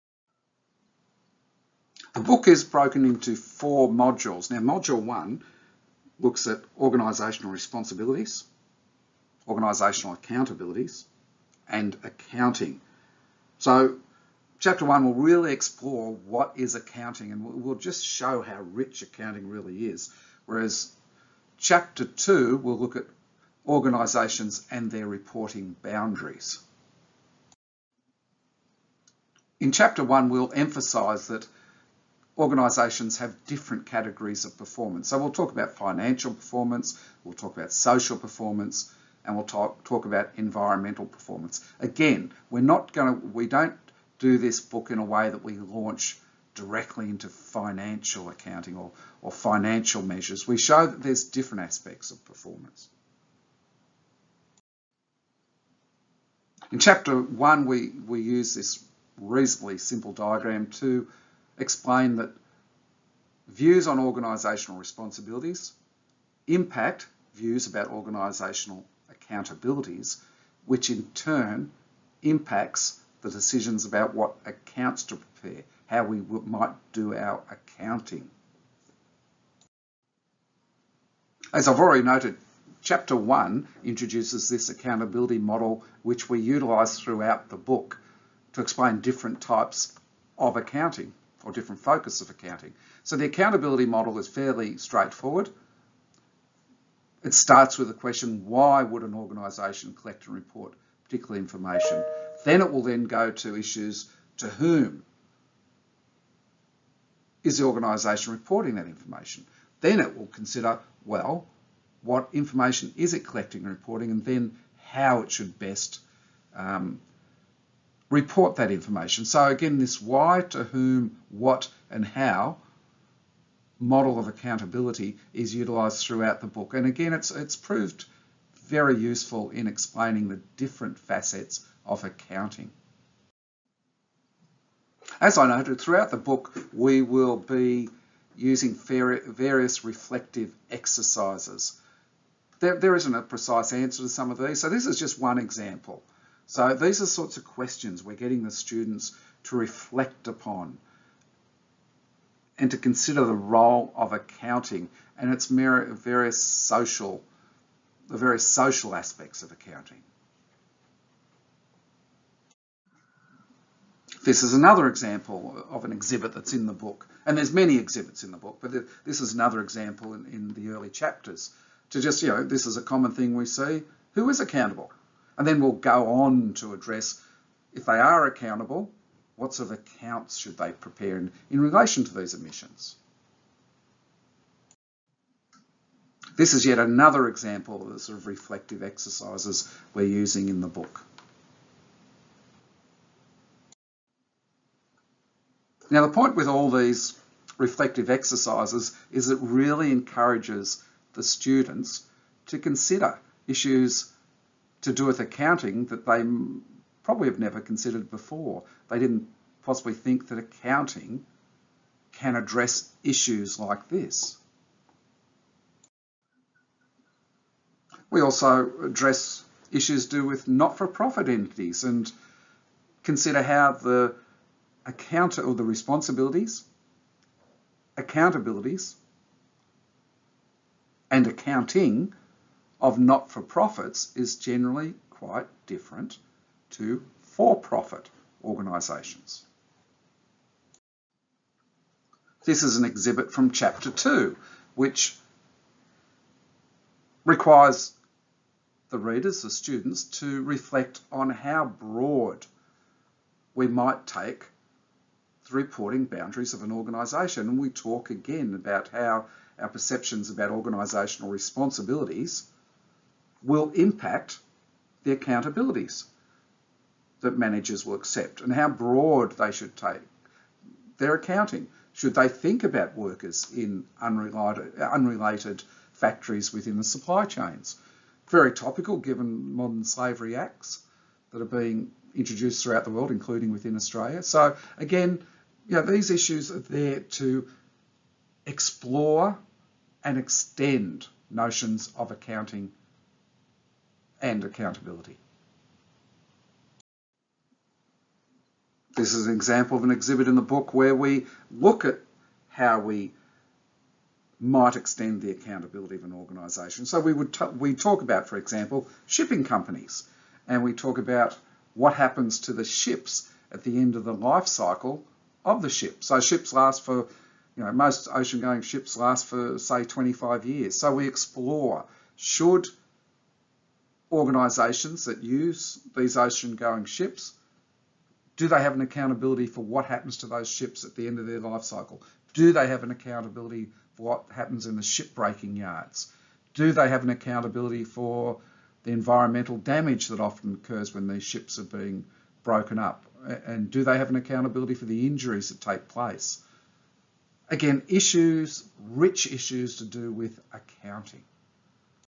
Approach and Features of Introduction to Accounting 1e with Craig Deegan
or ask your Cengage learning consultant for an inspection copy.
Inside this video, author Craig Deegan discusses:
0:00 Approach – Coverage of Chapters 1 & 2
1:21 The Accountability Model
2:14 Reflective Questions
4:01 Reporting Boundaries
4:58 Accountability of an Organisation

This is part two of a four part series.